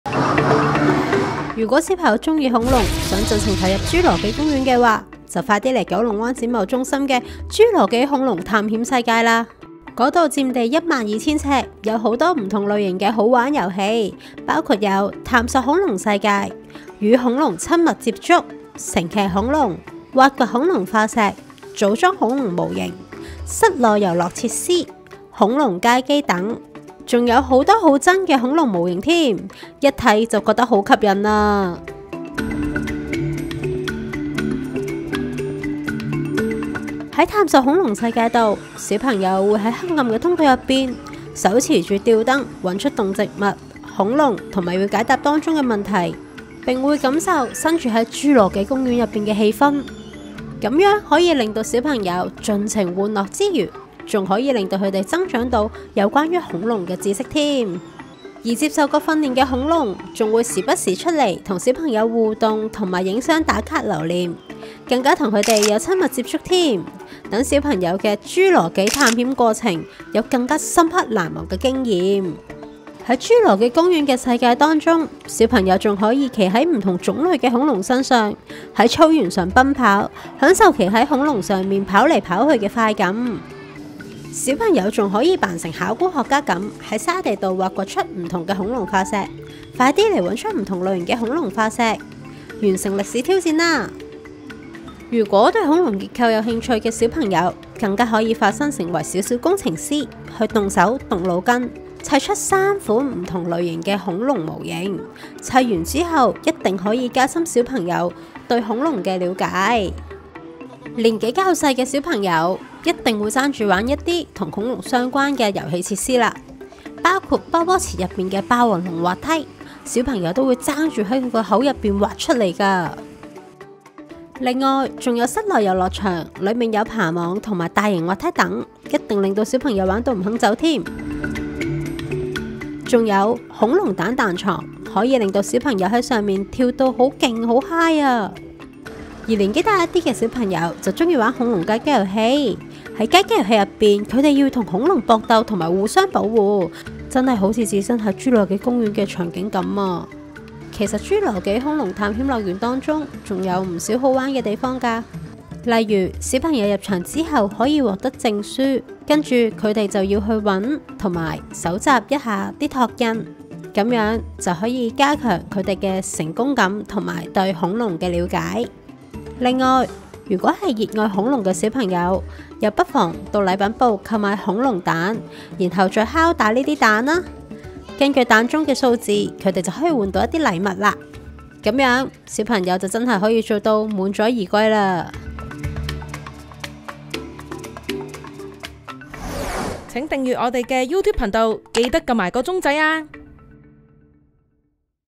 侏羅紀恐龍探險世界 刺激好玩 恐龍迷必去！
侏羅紀恐龍探險樂園 繼巡迴澳門、新加坡、馬來西亞及泰國後，即將於3月24日首次來到香港 九龍灣國際展貿中心B1 的 Let’s Play Entertainment Centre 舉行，讓 恐龍迷 好好感受一下 侏羅紀世界 的風采！

bmgproductionmusic